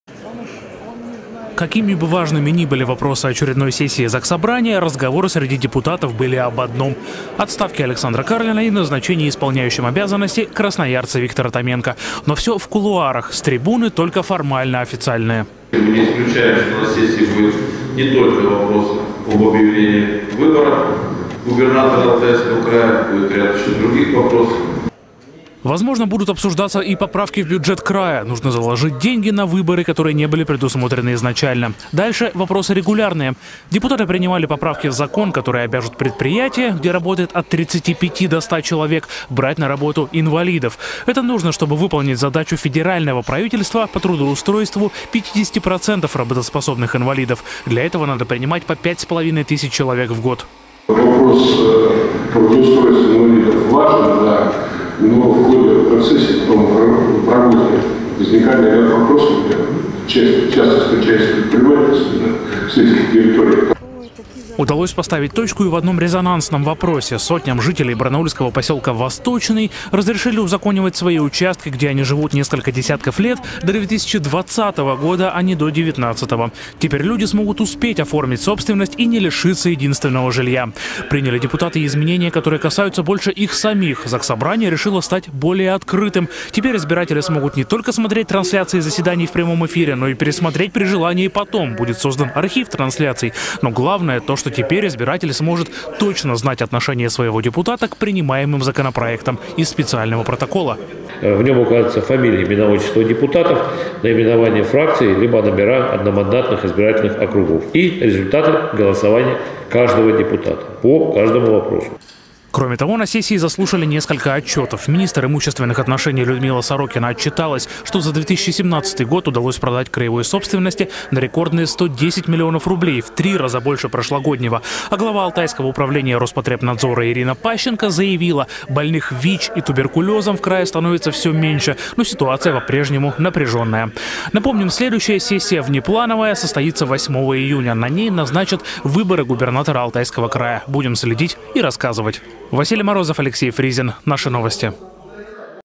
Депутаты смягчили требования к чиновникам сельских администраций
Главам сельских поселений теперь не обязательно быть профессиональными управленцами. На очередной сессии заксобрания депутаты смягчили требования к чиновникам сельских администраций. Это, считают в АКЗС, позволит привлечь к руководству свежие кадры.